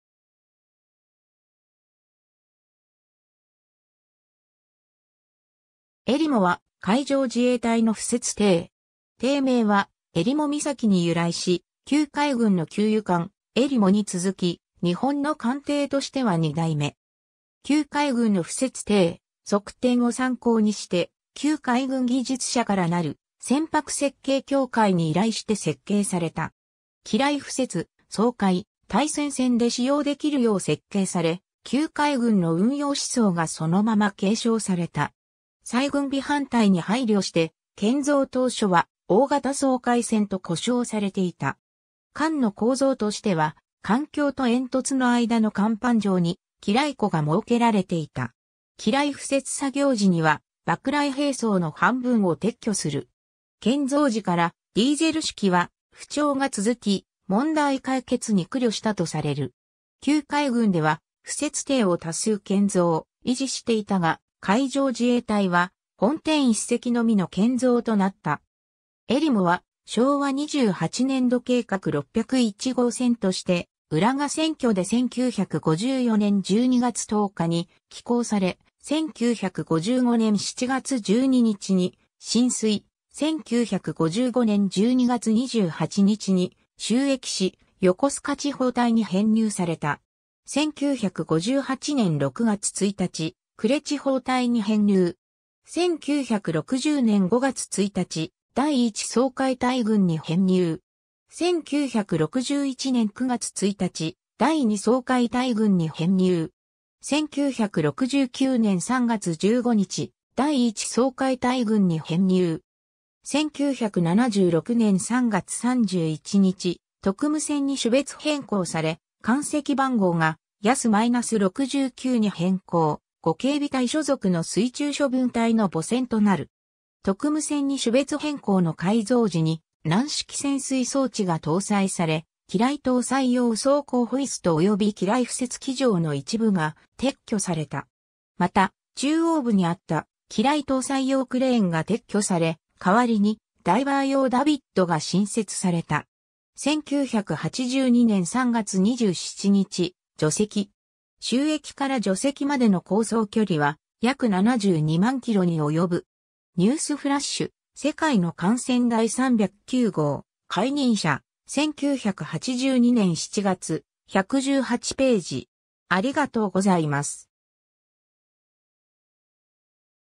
えりも (敷設艇)
えりも（ローマ字：JDS Erimo, AMC-491、YAS-69）は、海上自衛隊の敷設艇。
艇名は襟裳岬に由来し、旧海軍の給油艦「襟裳」に続き日本の艦艇としては2代目。
 旧海軍の敷設艇「測天」を参考にして旧海軍技術者からなる船舶設計協会に依頼して設計された。
 機雷敷設・掃海・対潜戦で使用できるよう設計され、旧海軍の運用思想がそのまま継承された。
再軍備反対に配慮して建造当初は大型掃海船と呼称されていた。
 艦の構造としては艦橋と煙突の間の甲板上に機雷庫が設けられていた。
機雷敷設作業時には爆雷兵装の半分を撤去する。
 建造時からディーゼル主機は不調が続き、問題解決に苦慮したとされる。
旧海軍では敷設艇を多数建造・維持していたが海上自衛隊は本艇1隻のみの建造となった。
 「えりも」は、昭和28年度計画601号船として、浦賀船渠で1954年12月10日に起工され、1955年7月12日に進水、1955年12月28日に就役し、横須賀地方隊に編入された。
 1958年6月1日、呉地方隊に編入。
 1960年5月1日、第1掃海隊群に編入。
 1961年9月1日、第2掃海隊群に編入。
 1969年3月15日、第1掃海隊群に編入。
 1976年3月31日、特務船に種別変更され、艦籍番号がYAS-69に変更、呉警備隊所属の水中処分隊の母船となる。
 特務船に種別変更の改造時に、軟式潜水装置が搭載され、機雷搭載用走行ホイスト及び機雷敷設軌条の一部が撤去された。
 また、中央部にあった機雷搭載用クレーンが撤去され、代わりにダイバー用ダビッドが新設された。
 1982年3月27日、除籍。
就役から除籍までの航走距離は約72万キロに及ぶ。
 ^ 「ニュース・フラッシュ」『世界の艦船』第309号、海人社、1982年7月、 118頁。